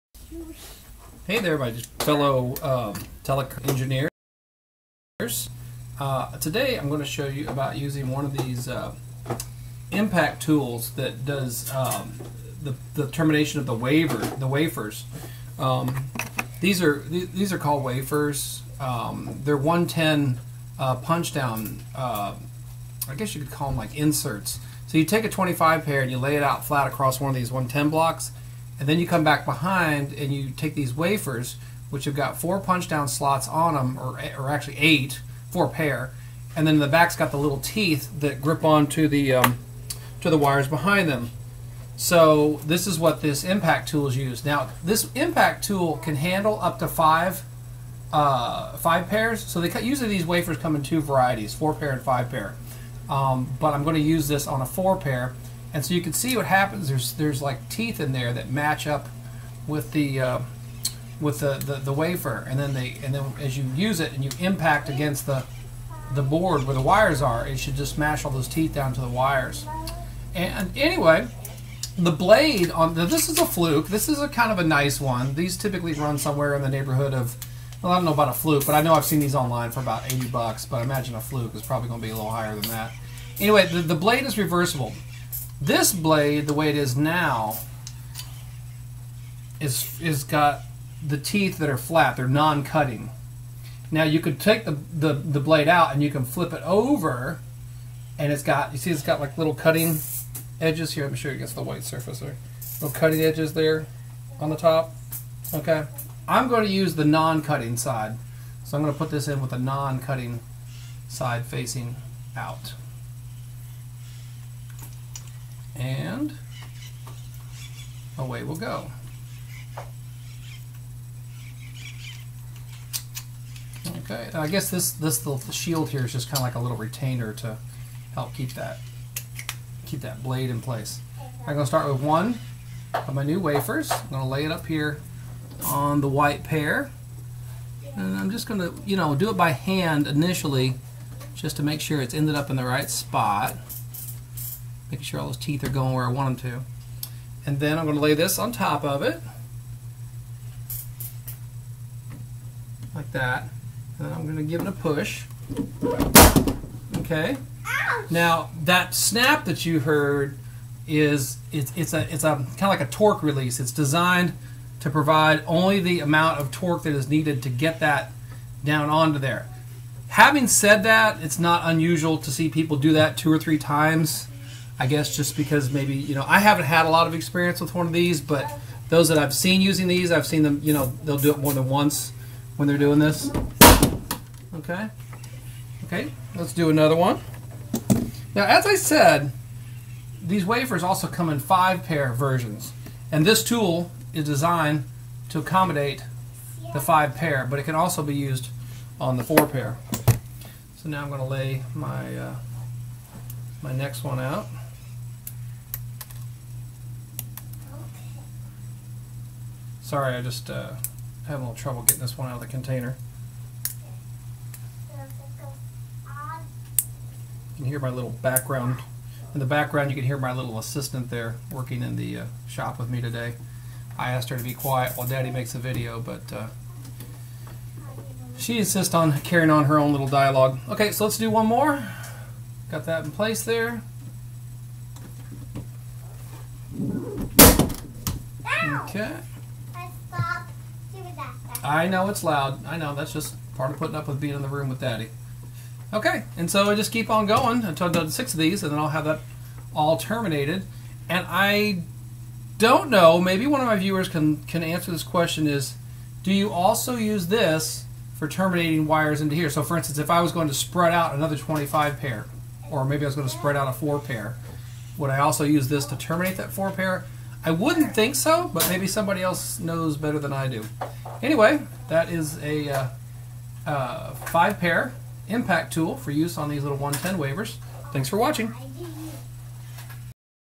5 pair punch tool or D-Impactor in action
This video briefly shows the use of a 5-pair impact tool for doing terminations on 110 blocks.

This tool terminates the 4 or 5 pair 110 wafers all at once.  Without this tool, you would traditionally have to use a 110 punch tool to do each wire at a time.

In the right hands and for the right situation, this cool could cut cable termination time in HALF!

Tool Fluke Networks  D-Impactor 5-pair Impact Tool (10059000) DOES NOT require a hammer.  Many that I have seen, require you hit the back with a hammer.  This one is spring loaded and doesn't require a lot of effort on the part of the technician.